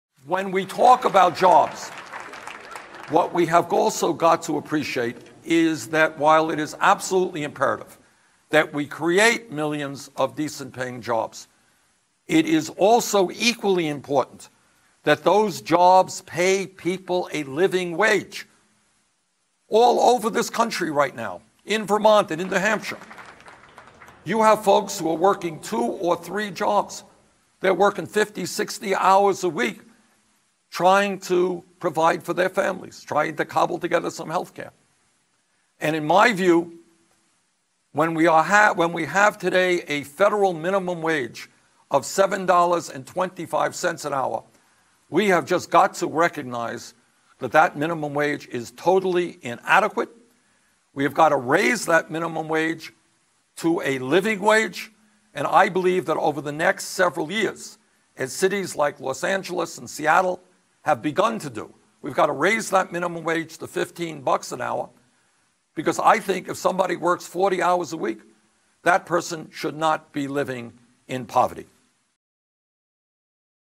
Bernie Sanders minimum wage should double to $15 an hour
Bernard "Bernie" Sanders (born September 8, 1941) is an American politician, serving as the junior United States Senator from Vermont since 2007. Sanders is the longest-serving independent in U.S. congressional history. He has always caucused with the Democratic Party, which has entitled him to committee assignments and at times given Democrats a majority. Sanders became the ranking minority member on the Senate Budget Committee in January 2015; he had previously served for two years as chair of the Senate Veterans' Affairs Committee. He publicly identified as a Democrat during his campaign for the party's 2016 Presidential nomination, but has announced he will return to the Senate as an independent.[3] A self-described democratic socialist, Sanders is pro-labor and emphasizes reversing economic inequality.[4][5] Many scholars consider his views to be more in line with social democracy and New Deal-style American progressivism than democratic socialism.